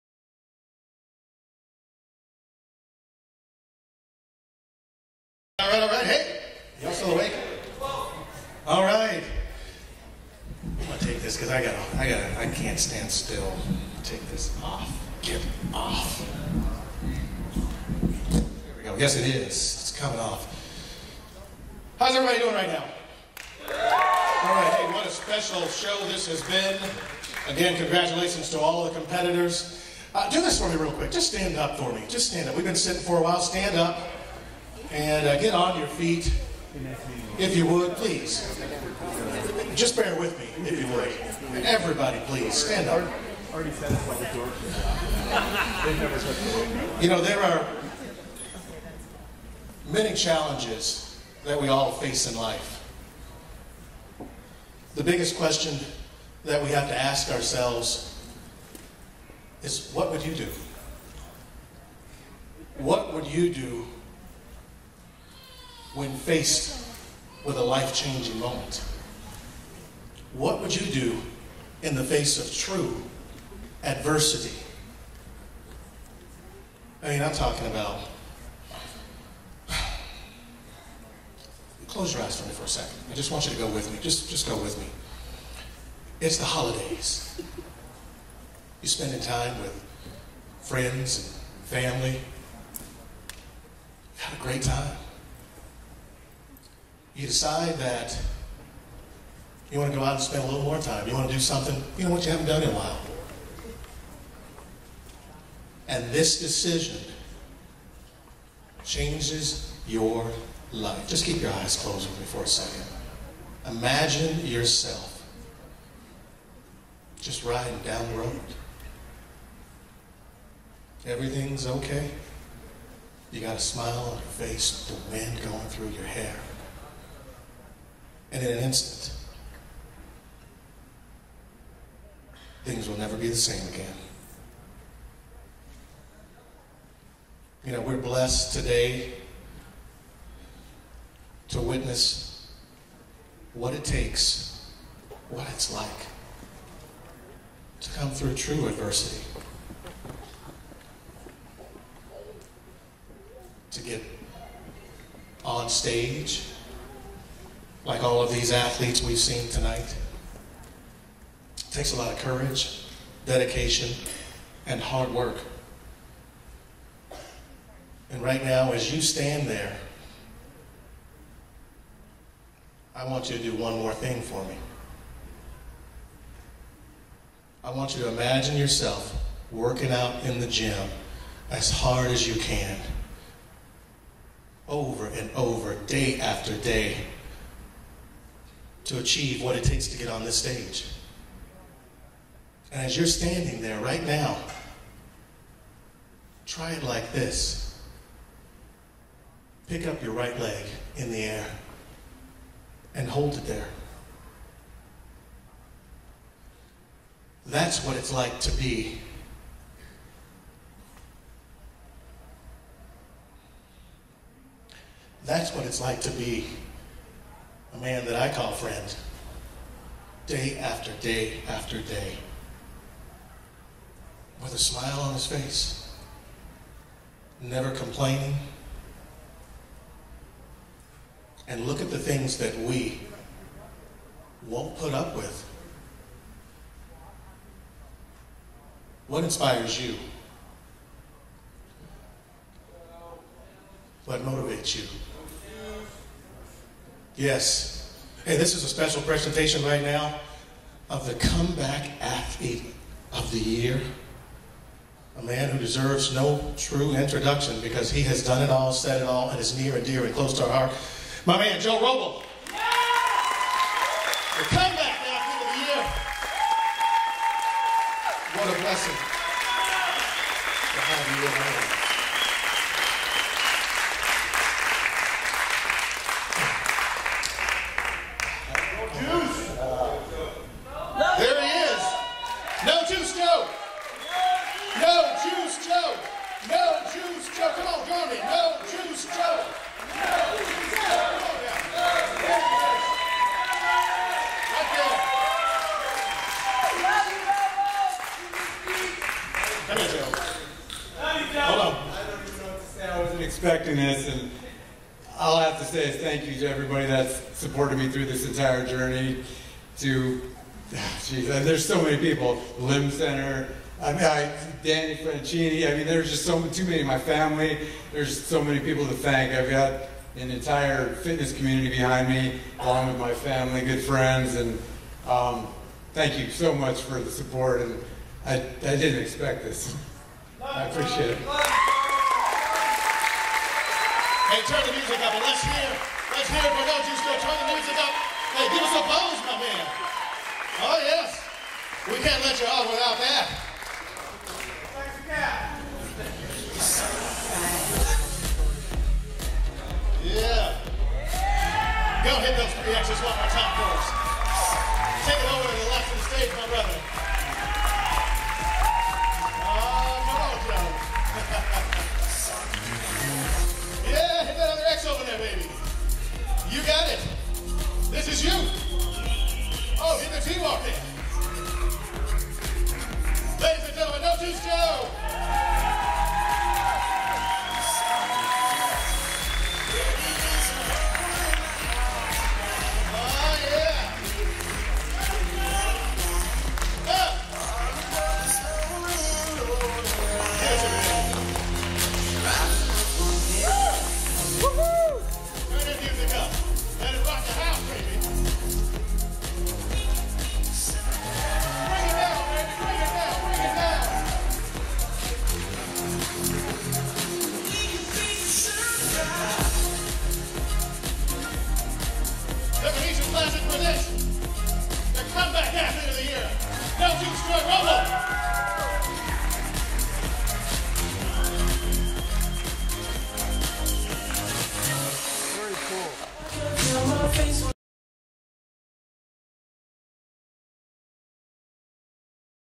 2017 INBF Phoenician Classic - Comeback Kid Award
The Comeback Kid Award.

Zig makes announce before giving award to Joesph ‘No Juice Joe’ Roybal